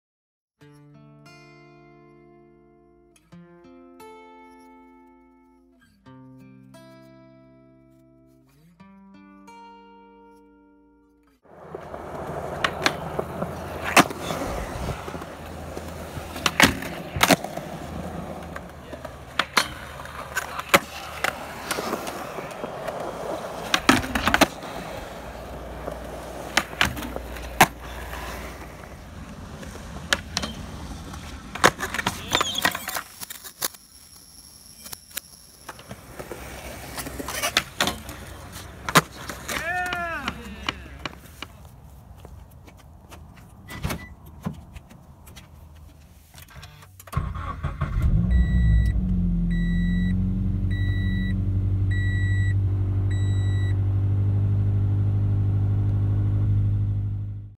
Front blunt kickflip out at Argonaut skatepark
Fun sesh at argonaut park and also relearning fs blunt bs flip out